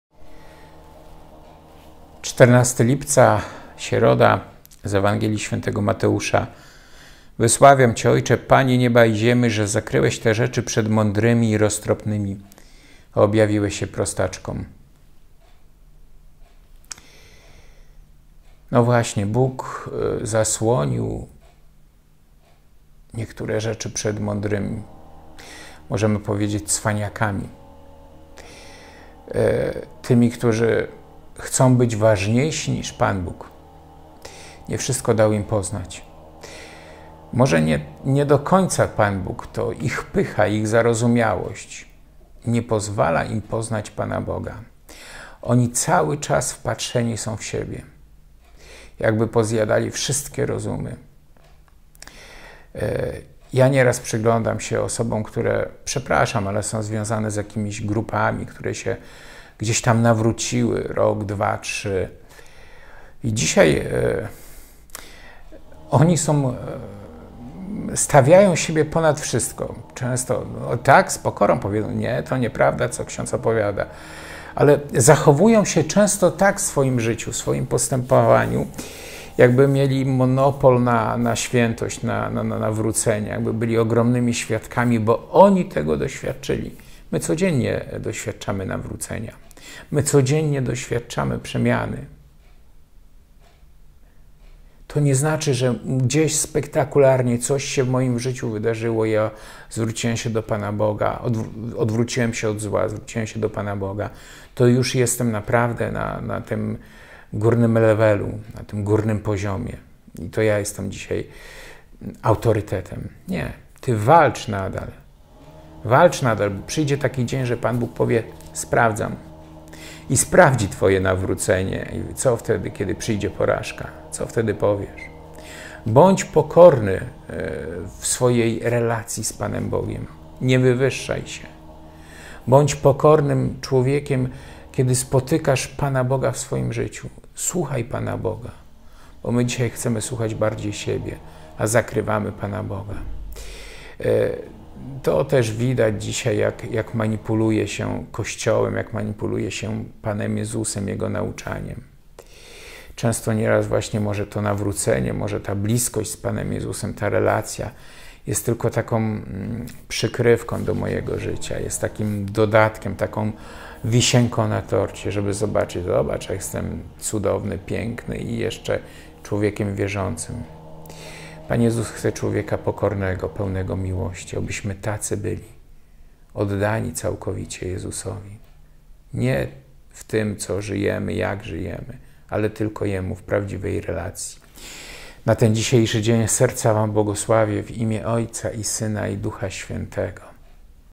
Środa - 14 lipca 2021r. Komentarz do Ewangelii, ks. Norbert Ostanek.
Słowa Ewangelii według Świętego Mateusza (Mt 11, 25-27)
W owym czasie Jezus przemówił tymi słowami: «Wysławiam Cię, Ojcze, Panie nieba i ziemi, że zakryłeś te rzeczy przed mądrymi i roztropnymi, a objawiłeś je prostaczkom. Tak, Ojcze, gdyż takie było Twoje upodobanie.
Wszystko przekazał Mi Ojciec mój. Nikt też nie zna Syna, tylko Ojciec, ani Ojca nikt nie zna, tylko Syn i ten, komu Syn zechce objawić».
Dziękuję. ❤️